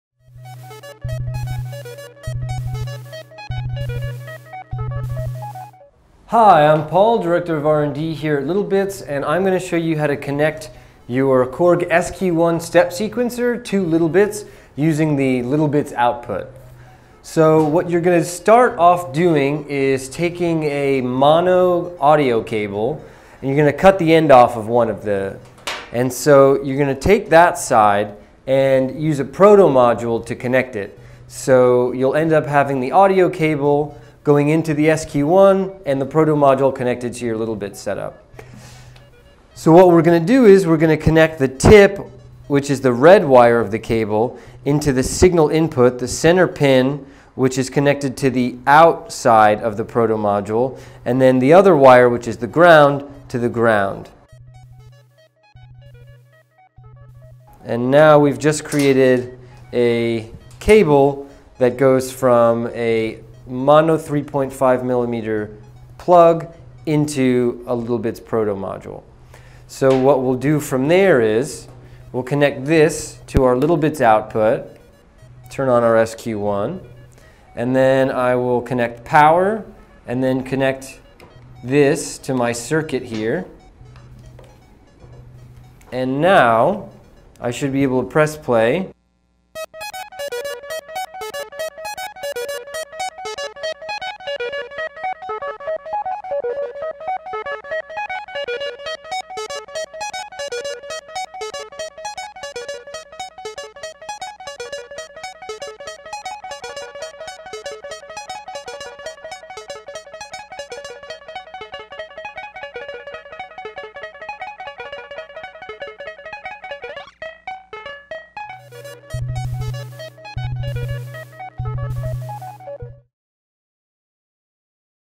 Sequencing the Synth Kit with the KORG SQ-1
To connect the SQ-1's littleBits OUT jack with the littleBits Synth Kit without the CV module, you'll need the littleBits Proto module and a mini-monaural cable.

1) Strip some insulation from the mini-monaural cable.

2) As shown in the illustration to the right, fasten the core and shield of the cable to the terminal screw of the Proto module.

3) Of the Proto module's three jumpers, remove the middle jumper.

4) Set up the littleBits circuit (one example is shown to the right), and connect the Proto module's mini-monaural cable to the SQ-1's littleBits OUT jack.

5) Press the SQ-1's START/STOP button to make the littleBits Synth Kit start playing.